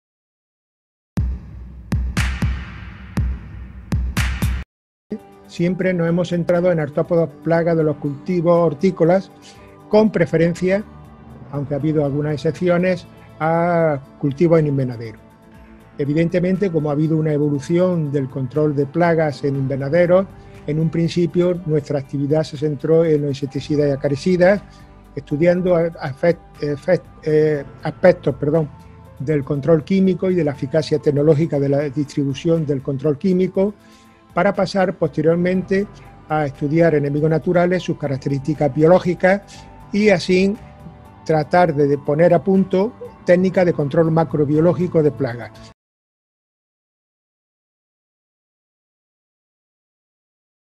El investigador del ceiA3 de la UAL Tomas Cabello, nos presenta el grupo AGR-107
El investigador del ceiA3 de la Universidad de Almería, Tomas Cabello García, nos presenta las líneas de investigación del grupo adscrito al ceiA3 AGR-107 "Protección vegetal de cultivos en invernaderos".